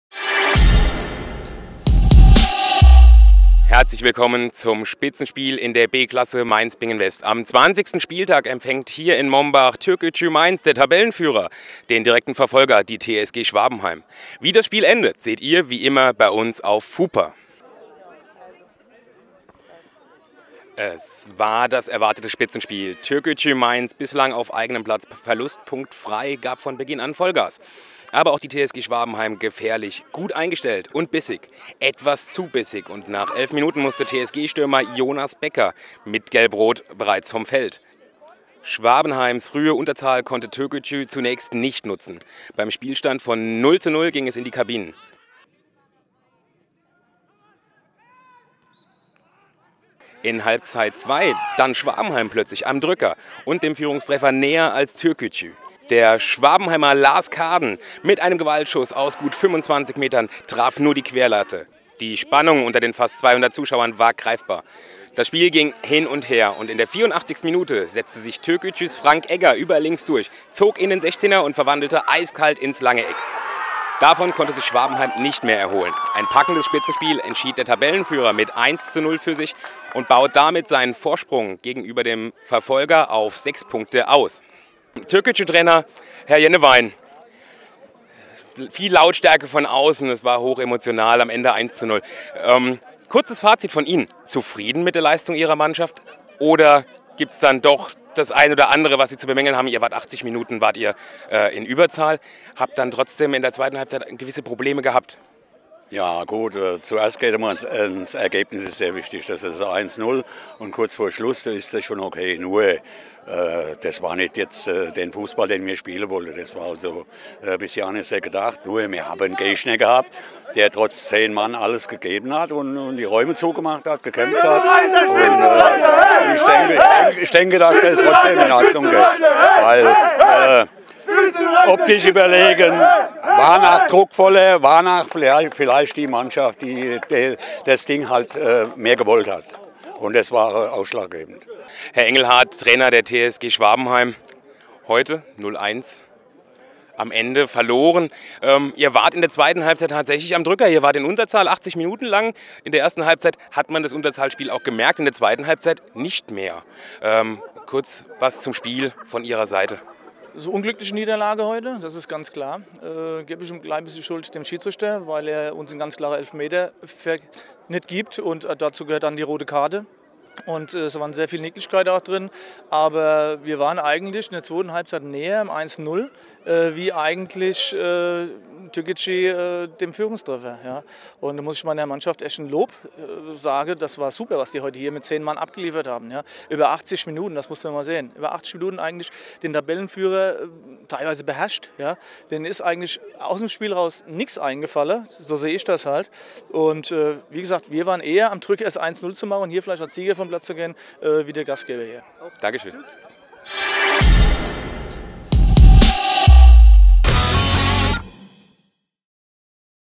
Spitzenspiel der B-Klasse Mainz-Bingen West
Der DITIB Türkgücü Mainz empfängt die TSG Schwabenheim zum Duell Eins gegen Zwei. Mehr zum Spiel und über FuPa gibt es